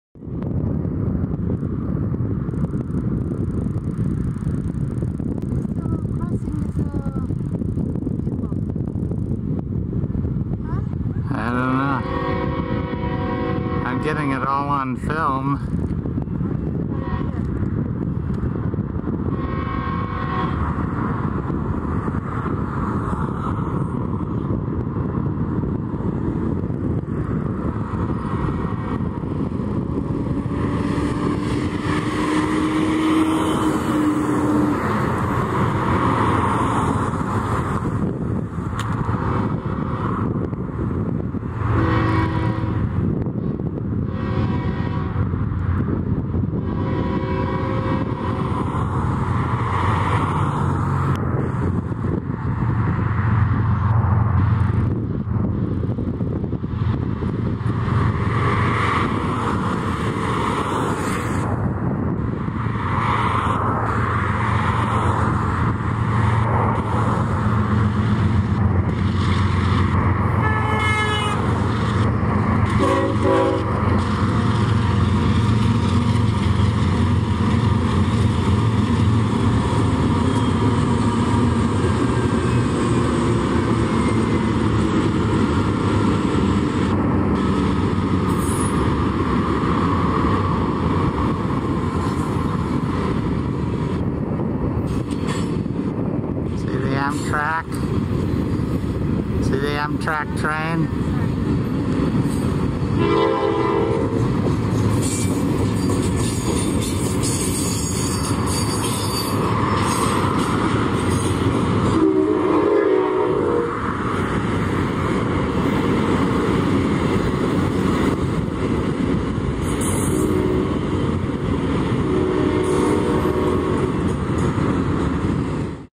BNSF intermodal: near accident and meet with Amtrak
As we were finishing our tour of Castle Air Force Museum, we noticed a potentially dangerous situation transpiring across the highway from us. Looks like an alert BNSF engineer saved the day.